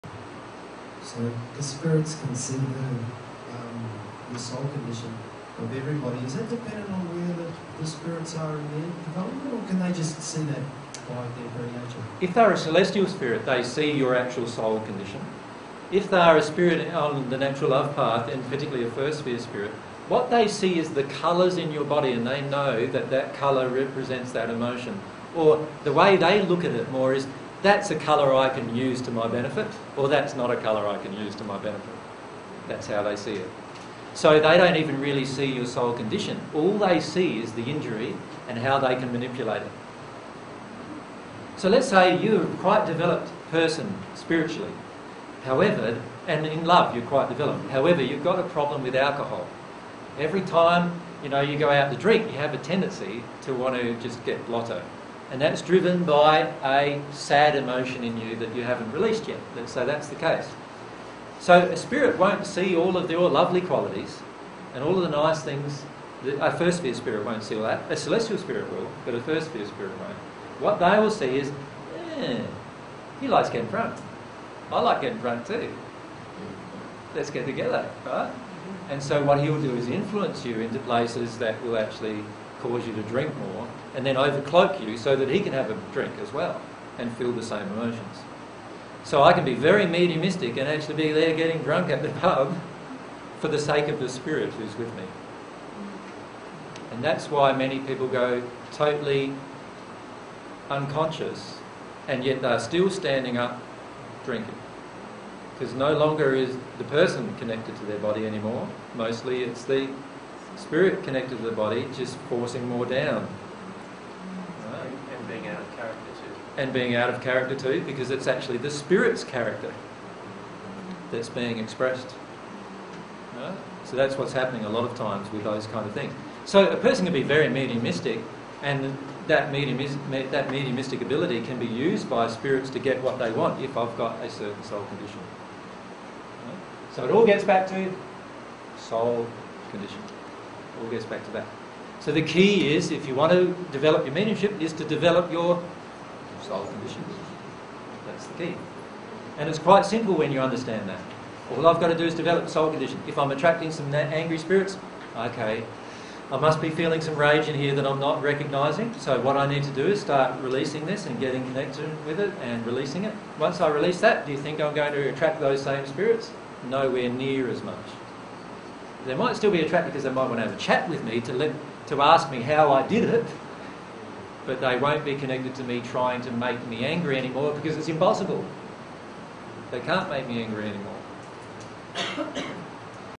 How Spirits Can Use a Medium Selfishly
Jesus talks about how spirits can use a medium's injuries to meet their selfish desires.

This is a section of the presentation by Jesus entitled "20090208 Spirit Relationships - The Law of Rapport & Spirit Communication P2".